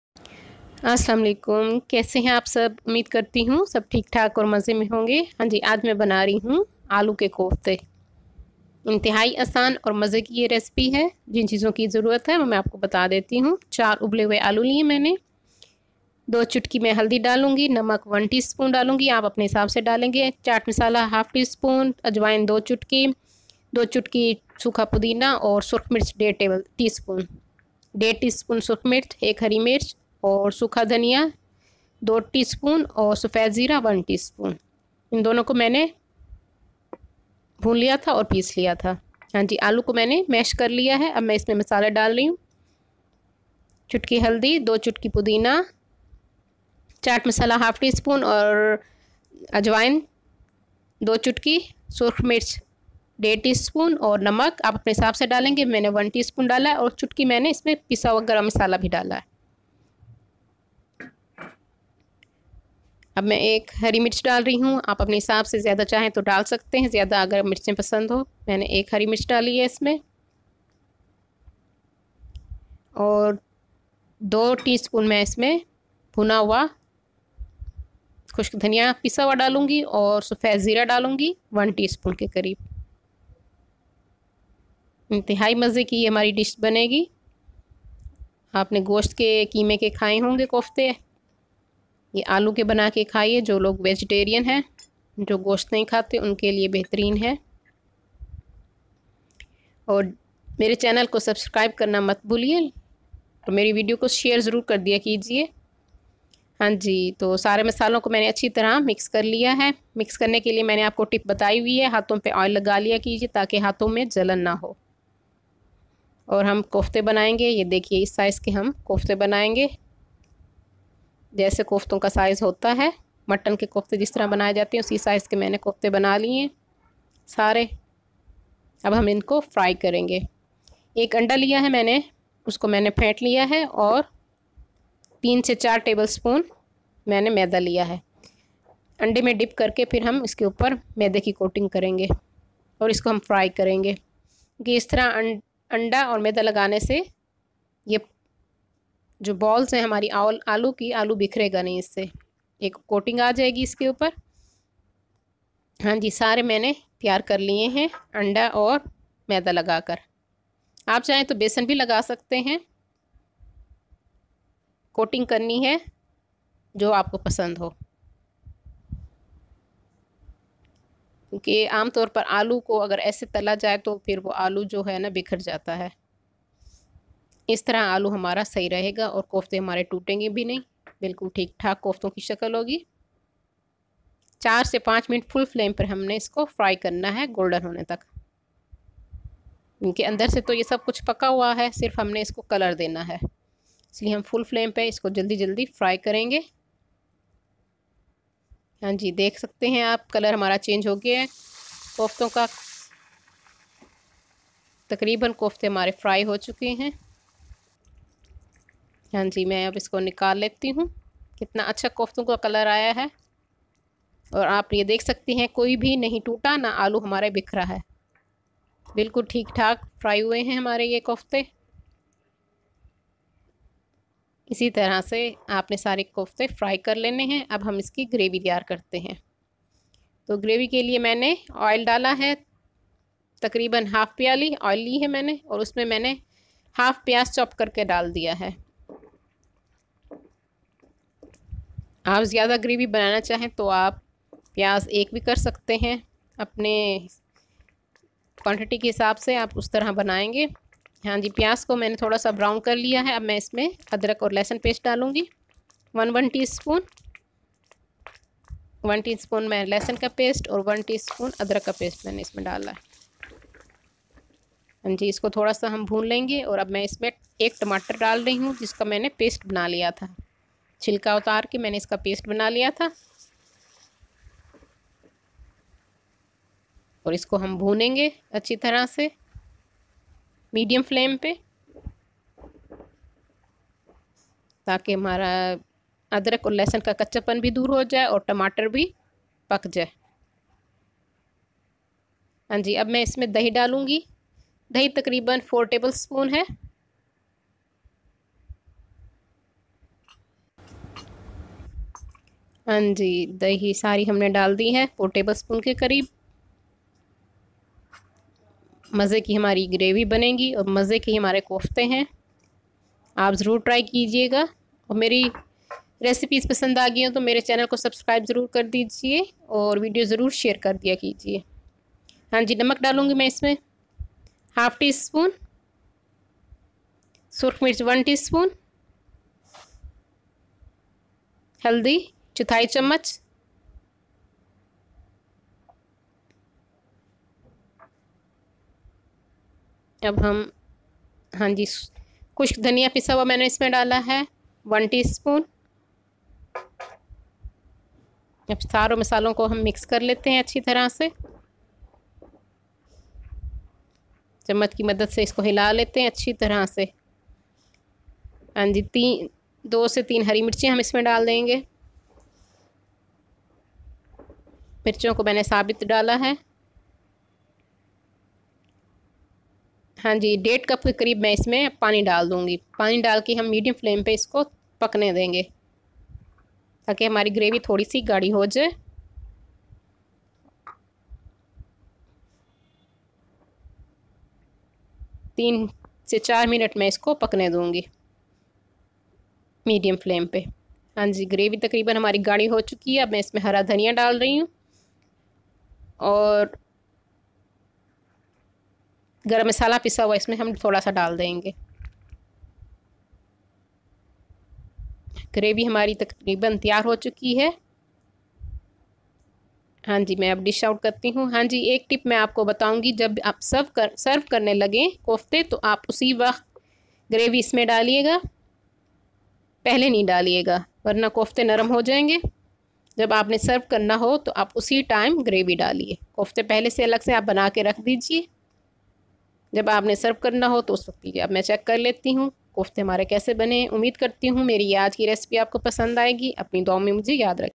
Aloo Ke Kofte ki Recipe. PotatoBalls آلو کے کوفتے کی ترکیب
4 آ لو ابلے ہوئے میش کر لیں اس میں ایک ٹی اسپون نمک۔ڈیرھ ٹی اسپون سرخ مرچ۔ آ رھا ٹی اسپون چاٹ مصالحہ۔2 چٹکی اجوائن۔2, چٹکی خشک پودینہ۔چٹکی ہلدی۔خشک دھنیا پاؤڈر بھنا ہوا 2 ٹی اسپون۔سفید زیرہ بھنا ہوا پسا ہوا ایک ٹی اسپون۔1_2,ہری مرچ چوپڈ ڈال کر مکس کریں اور کوفتے بنا لیں۔۔۔۔۔۔1انڈا پھینٹ لیں اس میں کوفتے ڈپ کریں اور میدہ لگا کر فل آ نچ پر 3_4 منٹ تک فرائی کریں۔۔۔۔۔۔۔ گریوی کے لیے آ رھا درمیانہ سائز کا پیاز پیس کر آ دھی پیالہ تیل میں ہلکا براؤن کریں اور پھر ایک ٹی اسپون ادرک اور ایک ٹی اسپون لہسن ڈال دیں تھوڑا بھون کر ایک ٹماٹر کی پیسٹ ڈال دیں پھر 4_5 چمچ دہی ڈال دیں ۔اور نمک 1/2 چمچ۔سرخ مرچ ایک چائے کا چمچ۔دو چٹکی ہلدی اور ایک ٹی اسپون دھنیا پاؤڈر ڈال دیں تھوڑا بھون لیں پھر ڈیڑھ کپ پانی ڈال دیں 3 منٹ ہلکی آنچ پر پکائیں۔تھوڑا گاڑھا ہونے پر اتار لیں۔۔۔ جب سرو کریں تو گریوی میں آ لو کے کوفتے ڈال دیں۔